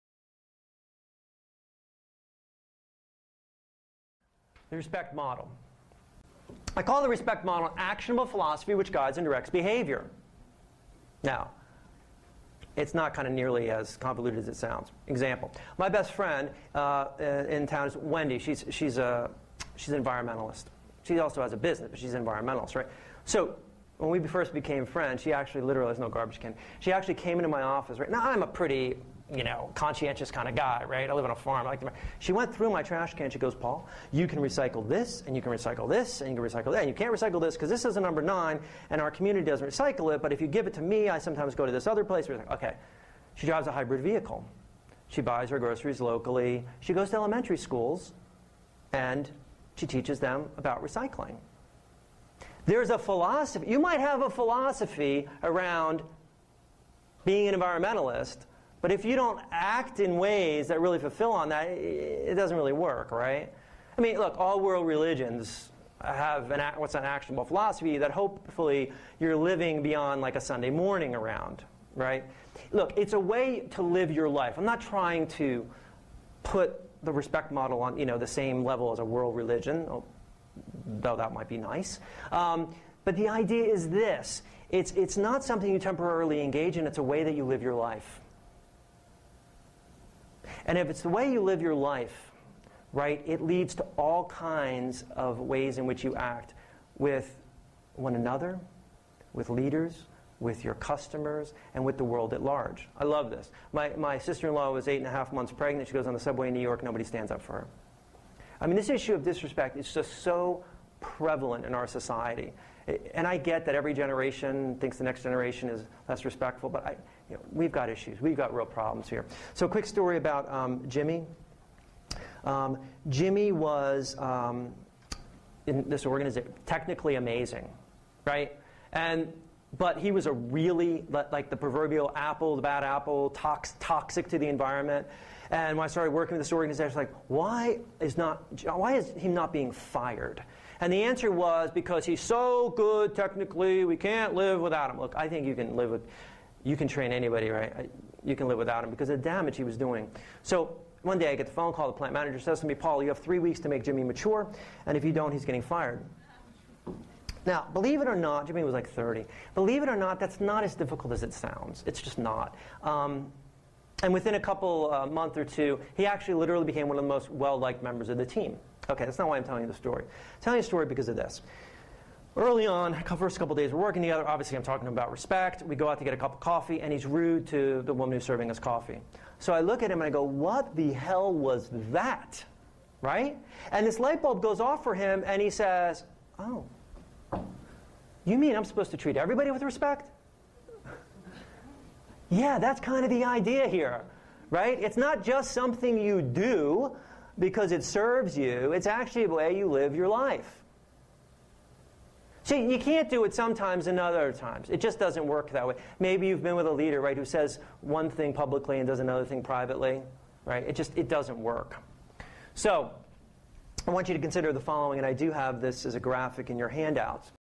Paul Marciano - The Respect Model
Dr. Paul Marciano describes The Respect Model as an actionable philosophy which guides and directs behavior